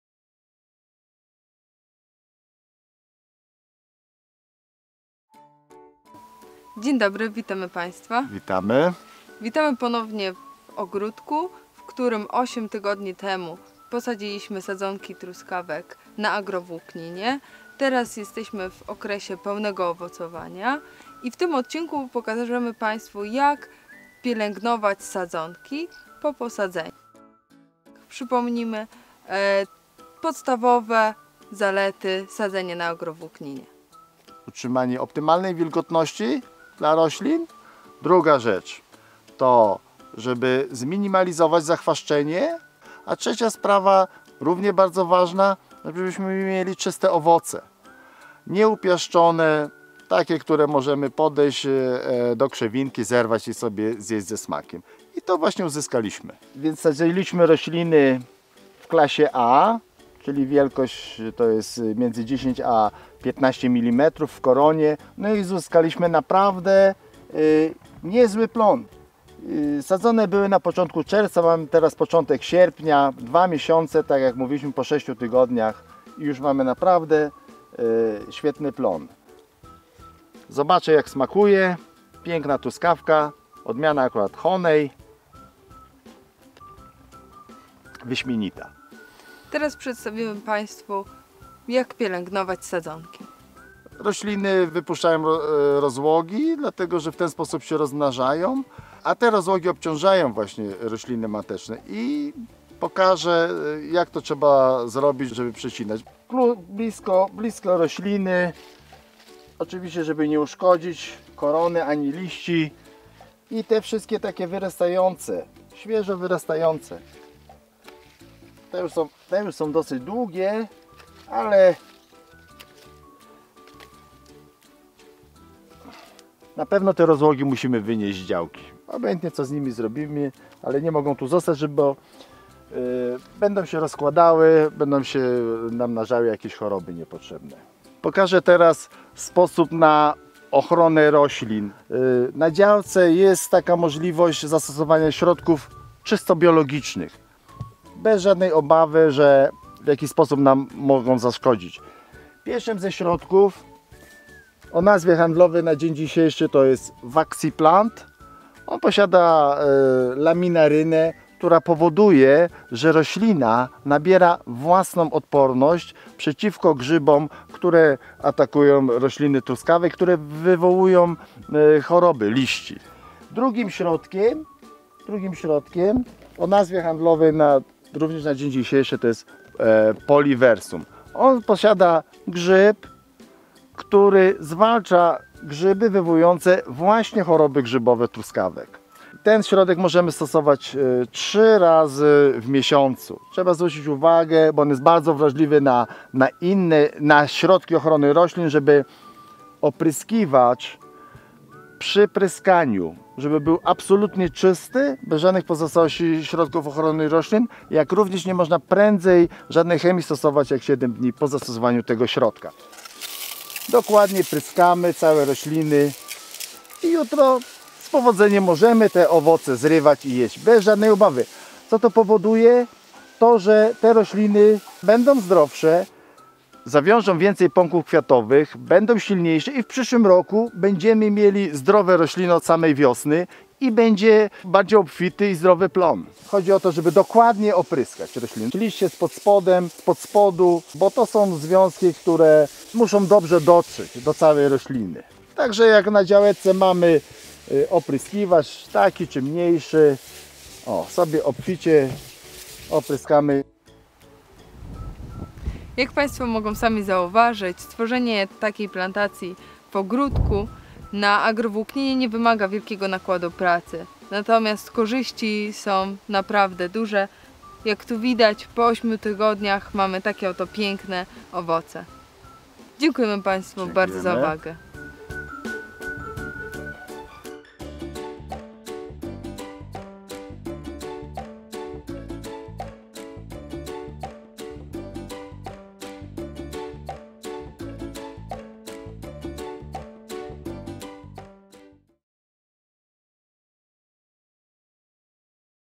Pielęgnacja truskawek na agrowłókninie - Poradnik dla ogrodników
Krótko, fachowo i przystępnie dla każdego amatora własnych truskawek:
1. Dlaczego warto uprawiać truskawki na agrowłókninie
2. Dlaczego i jak przycinać pędy, które wypuszczają truskawki
3. Jak ekologicznie chronić truskawki
4. Plony z uprawy truskawek Frigo na agrowłókninie